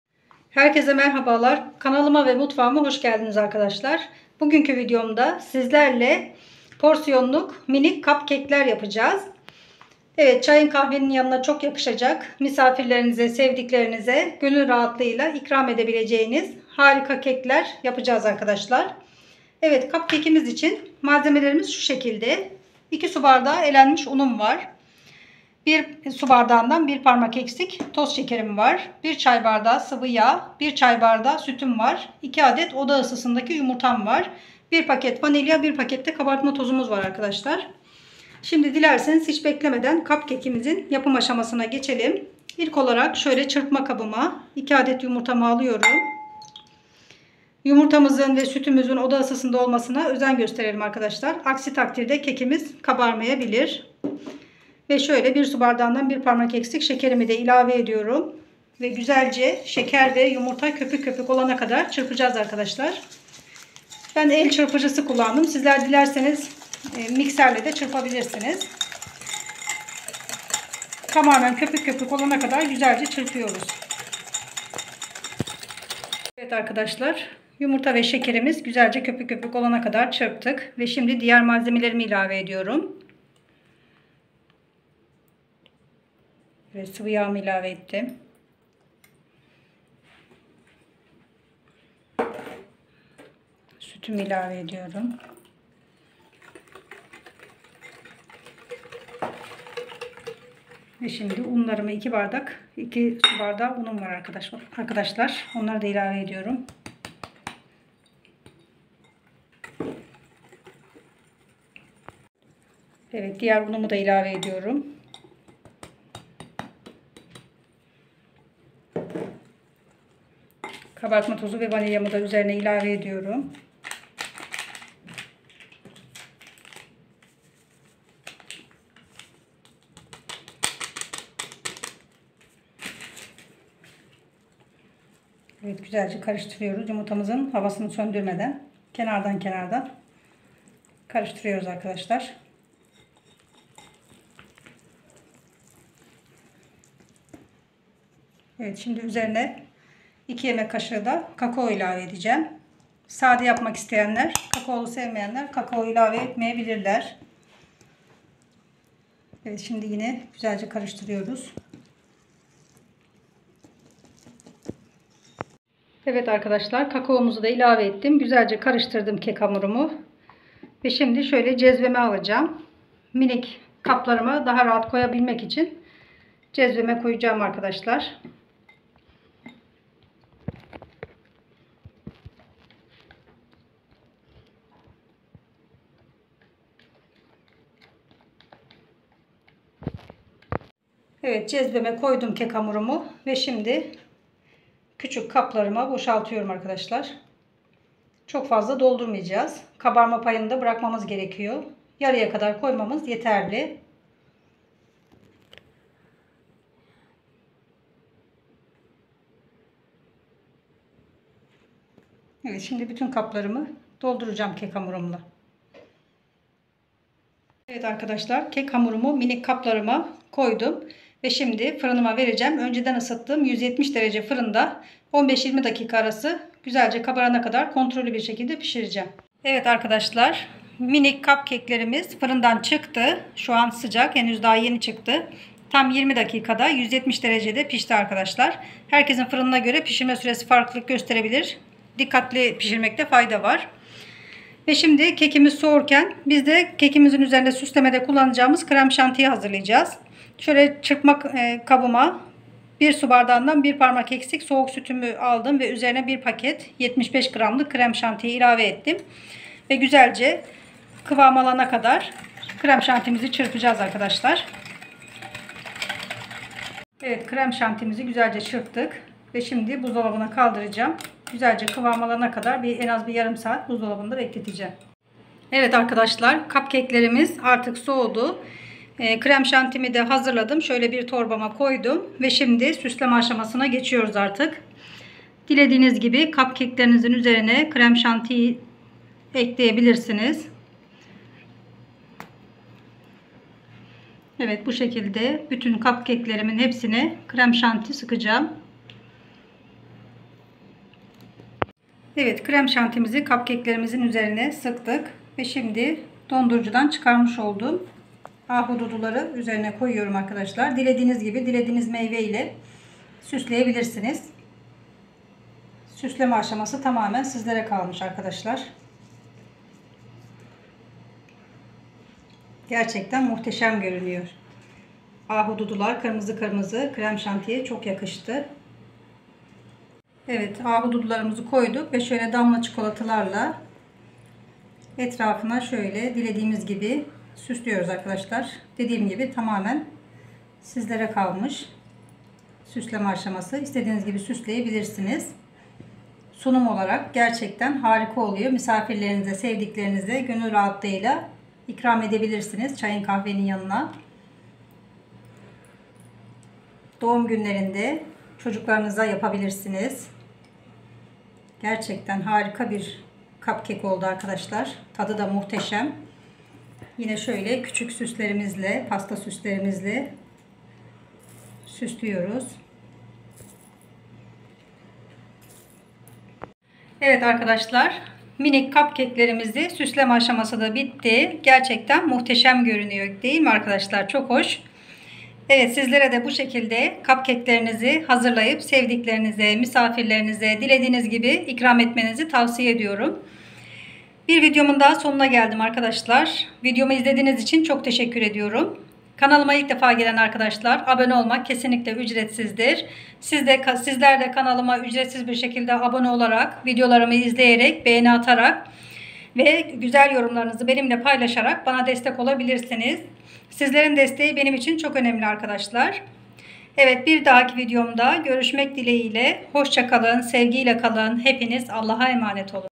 Kakaolu Cupcake Tarifi / Krem Şantili Çikolatalı Cupcake Nasıl Yapılır?
Merhabalar arkadaşlar bugün sizlerle kakaolu Cupcake tarifi paylaştım gerçekten muhteşem oldu sizlerde misafirlerinize sevdiklerinize porsiyonluk harika Cupcake hazırlayabilirsiniz hem lezzeti hem de görüntüsü ile herkesin övgüsünü kazanabilirsiniz deneyecek olanlara şimdiden afiyet olsun

       Malzemeler
2 adet yumurta ( oda ısısında)
1 su bardağından bir parmak eksik şeker
1 çay bardağı sıvı yağ
1 çay bardağı süt
2 su bardağı un (elenmiş)
2 yemek kaşığı kakao
1 paket kabartma tozu
1 paket vanilya

süsleme için
1 su bardağından bir parmak eksik süt ( soğuk)
1 paket krem şanti ( 75 Gr )
damla çikolata